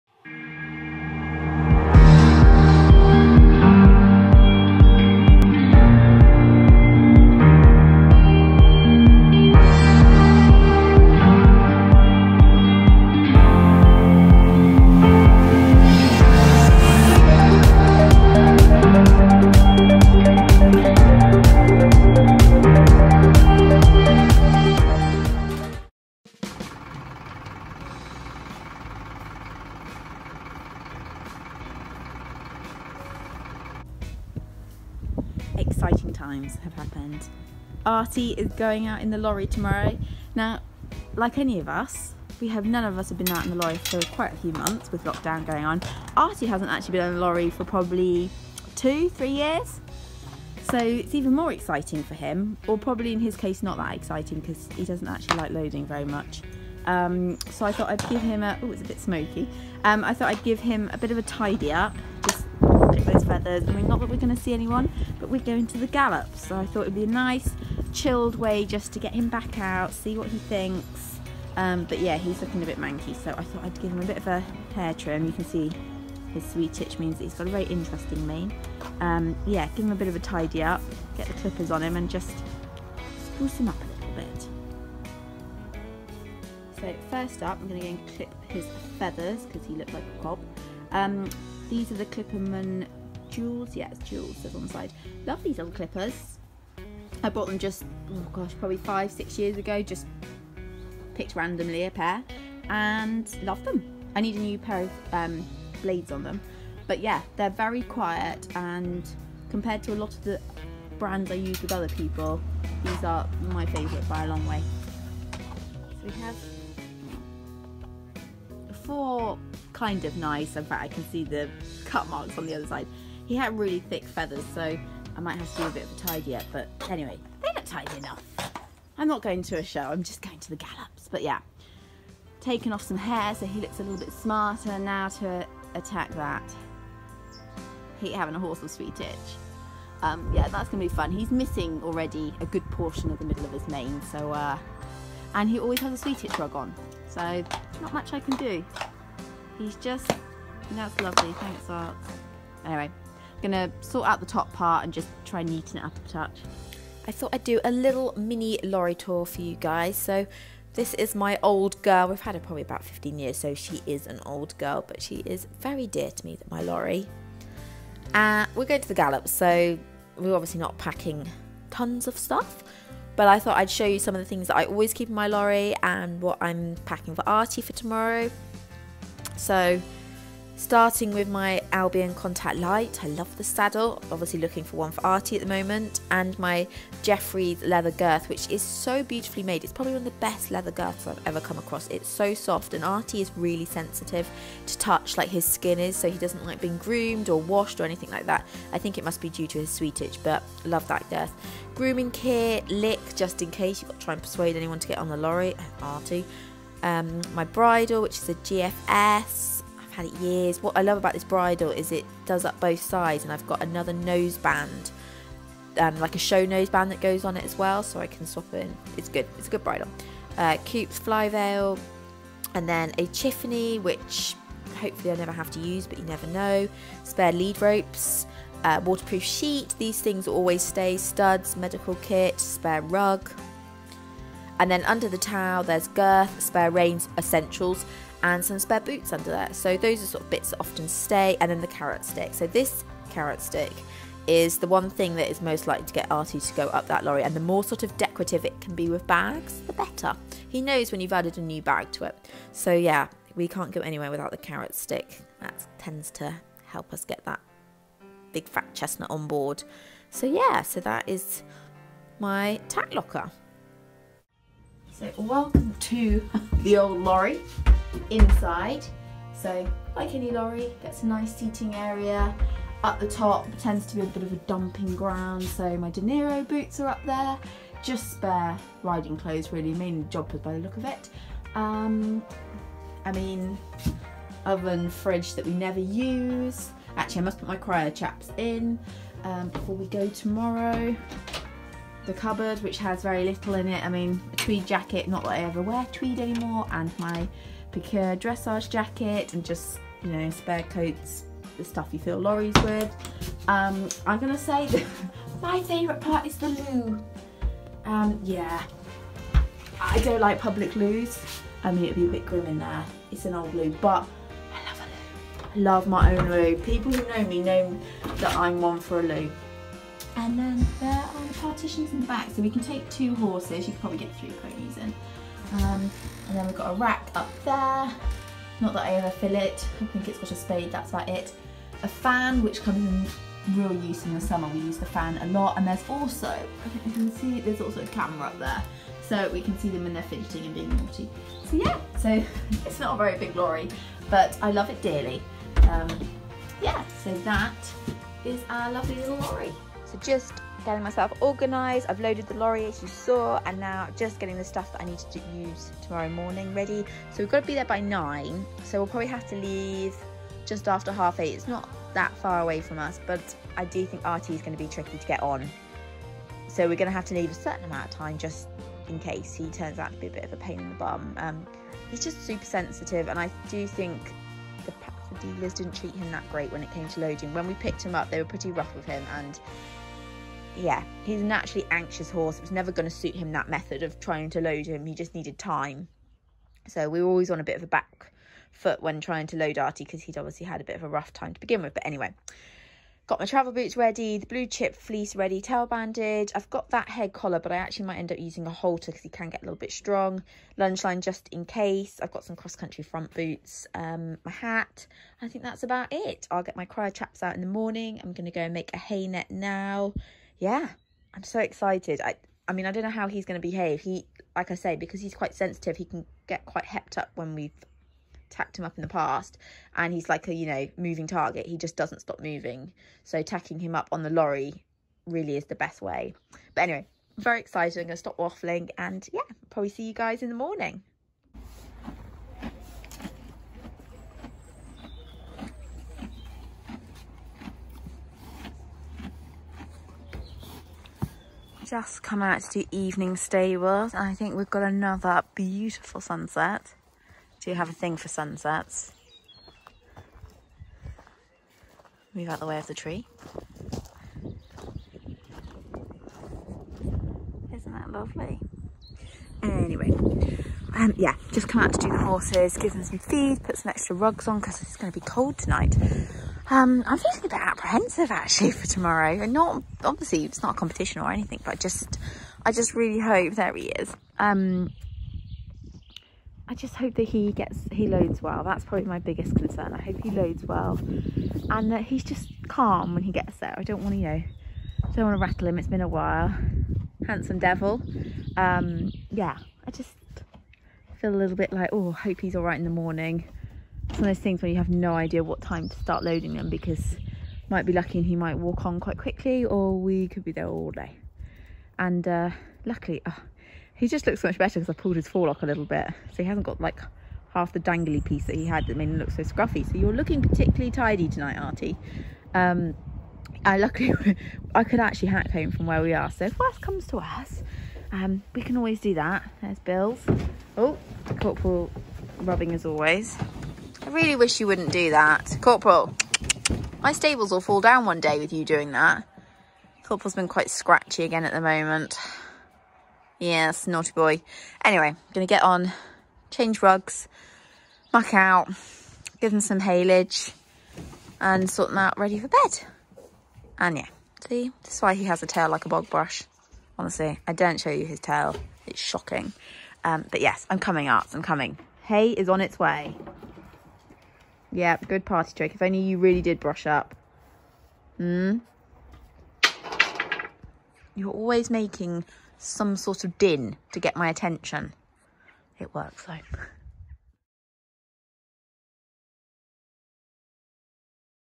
HORSE BOX TOUR | Equestrian Vlog
Finally the old girl (horse-box) is back out and ready for an outing! Thought I'd do a quick lorry tour, and get Arty ready for his first outing in the lorry in over 2.5 years!!